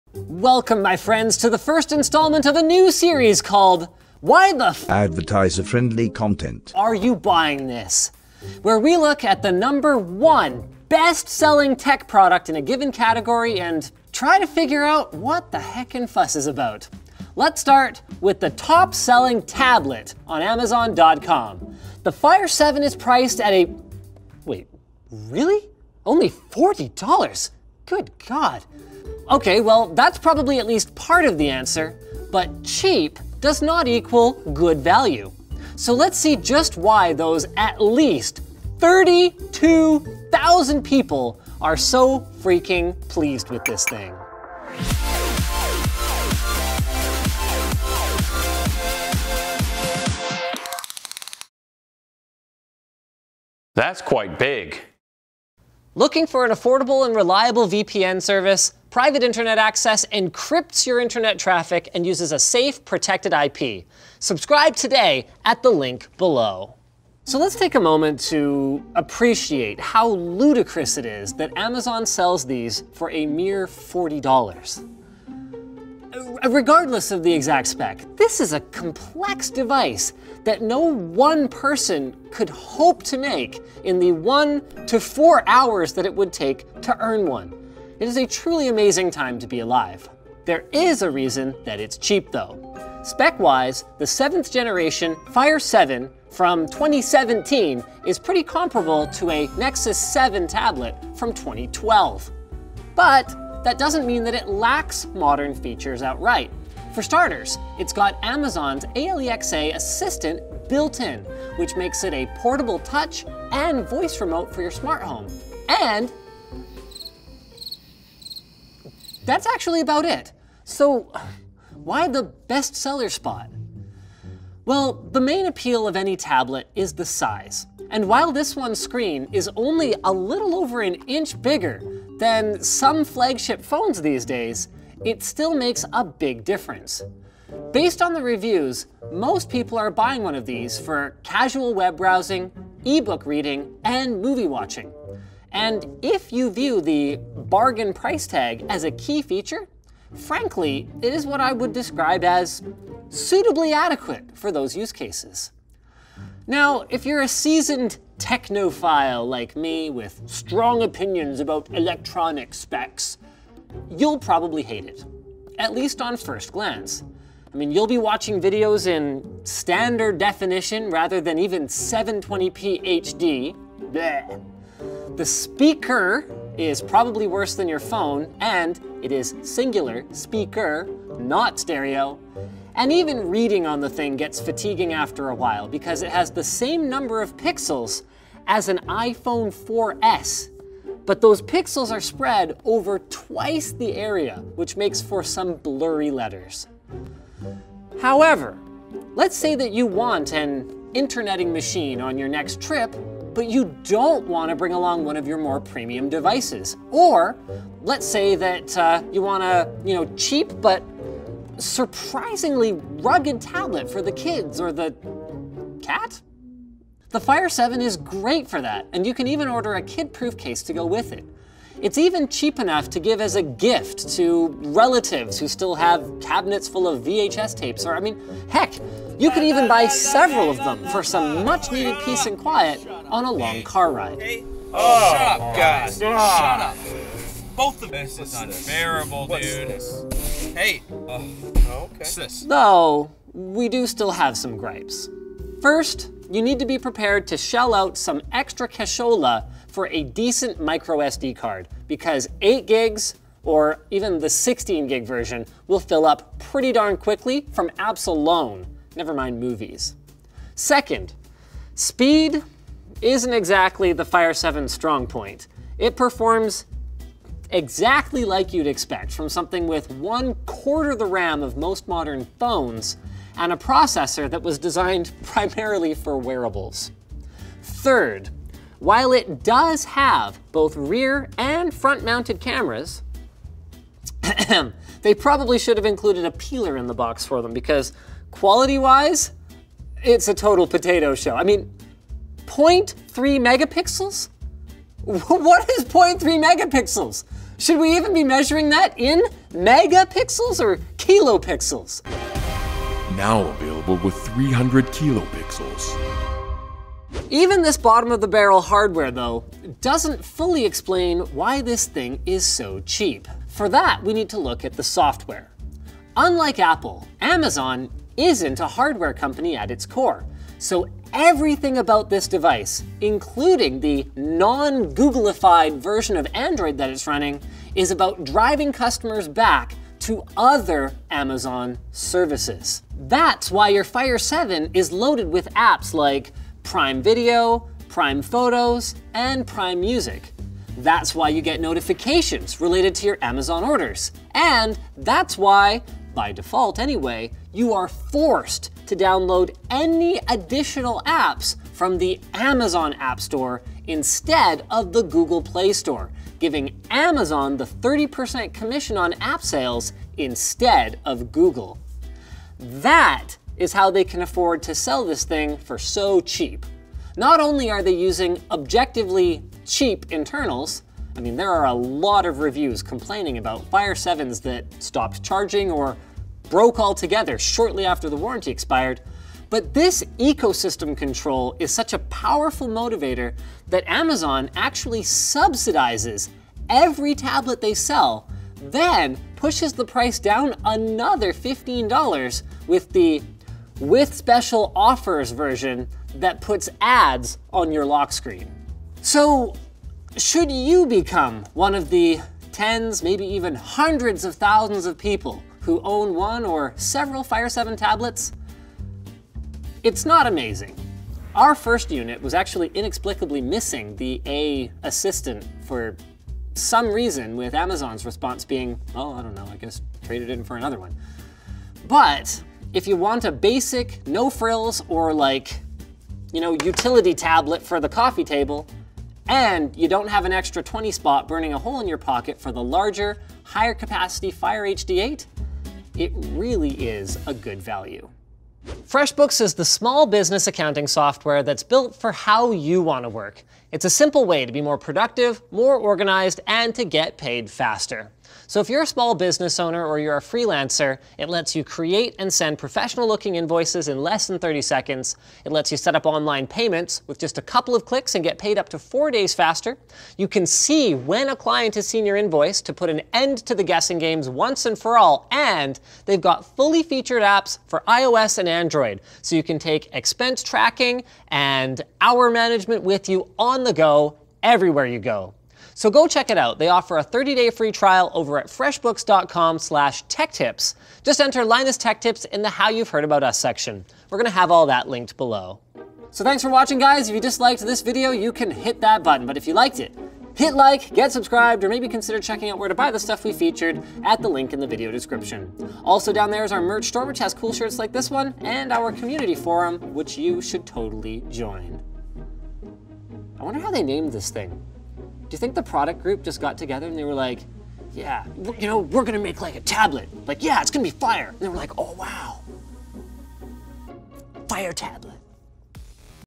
Why is EVERYONE Buying this Tablet?? - Amazon Fire 7
Buy Fire 7 tablet:
On Amazon
On Newegg

It may not be HD.. It doesn't have the best battery life.. But the Amazon Fire 7 is definitely the best tablet you can get (for $40) so why not? It does Netflix, it has a headphone jack, what more do you want? ( ͡° ͜ʖ ͡°)